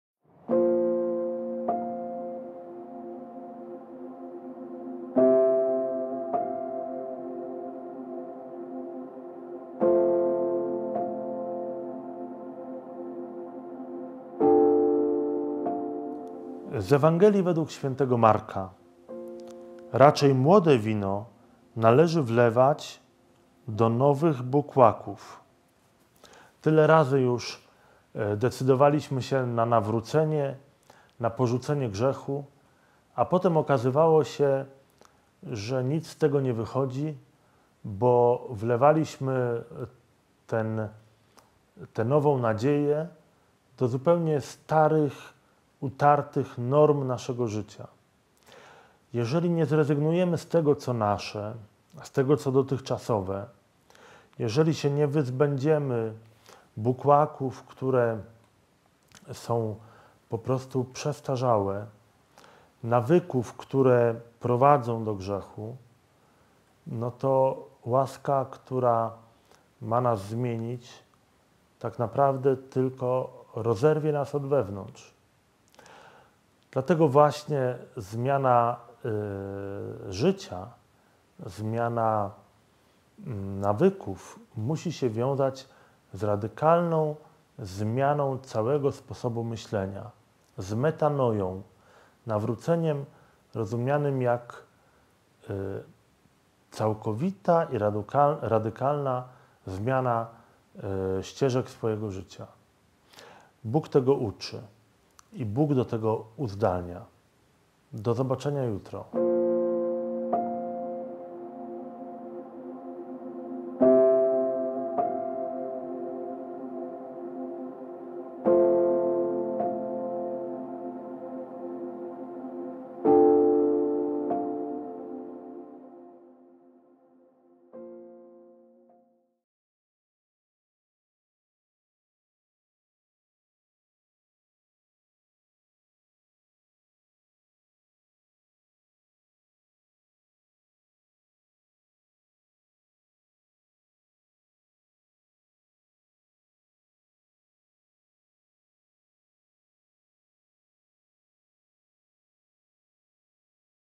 Kilka słów o Słowie 21 I 2019
Mój komentarz do Ewangelii na poniedziałek, 21 stycznia 2019
Zachęcam, by przeczytać najpierw fragment Ewangelii Mk 2, 18-22